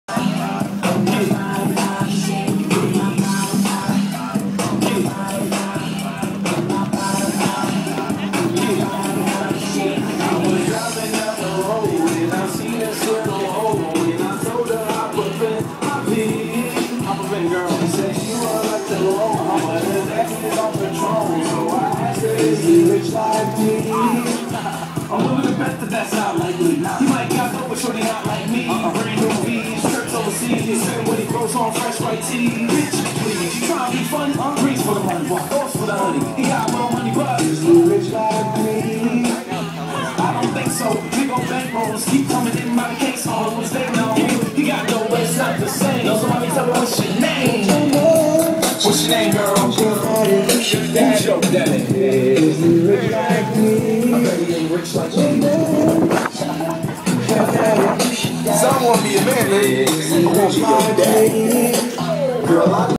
Old Man Dances Like LMFAO
Venice is a crazy place.  Just say no to drugs, kids---Let's put 30 seconds on the clock and see how many comments we can come up with...Ready?  GO!